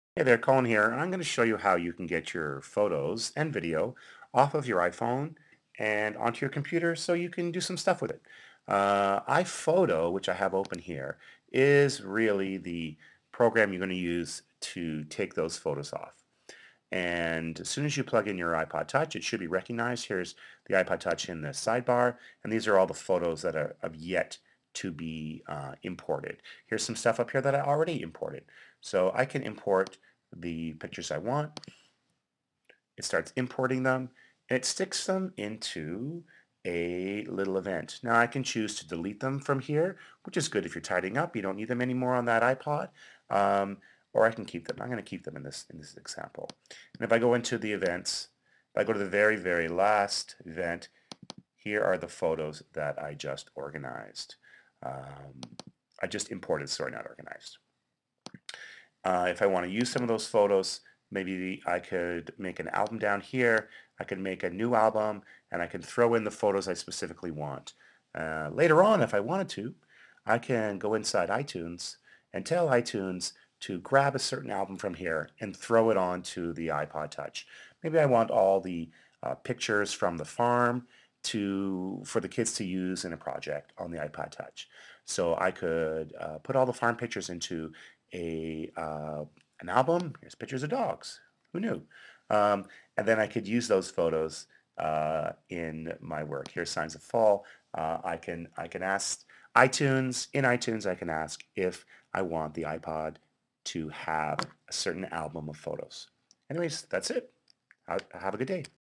Transfer photos and videos from your ipod touch/iPad to your MAc via
Here's how to upload you photos and videos to iPhoto.